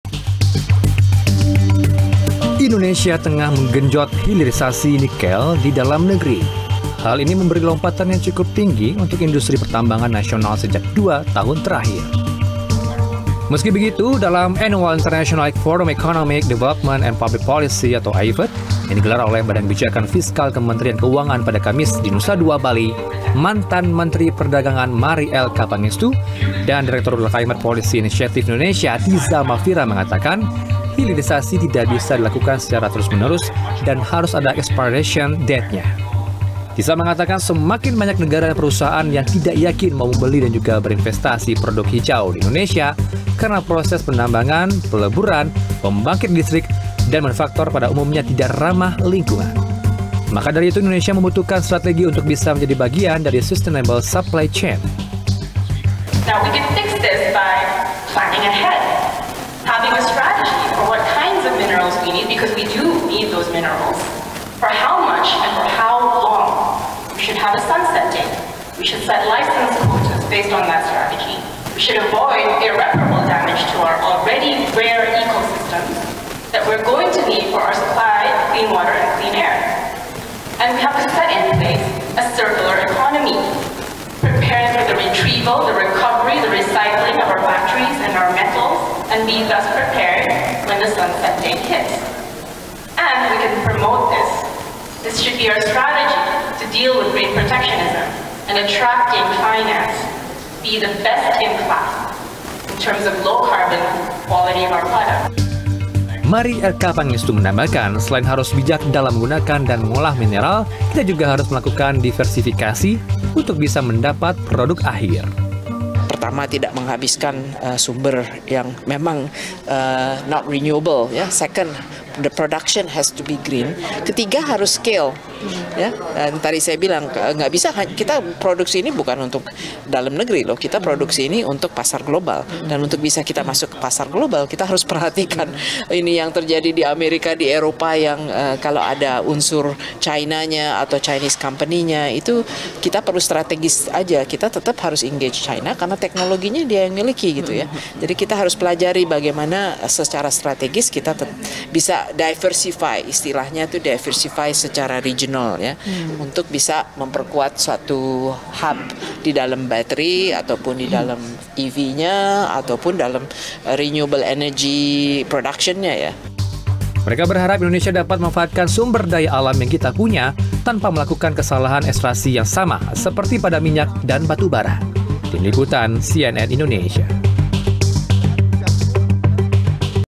Hilirisasi Harus Ramah Lingkungan
Hilirisasi merupakan upaya yang telah dilakukan untuk meningkatkan nilai tambah komoditas yang dimiliki.
Namun, Indonesia harus bisa memanfaatkan sumber daya alamnya dengan bijak, tanpa merugikan negara sendiri.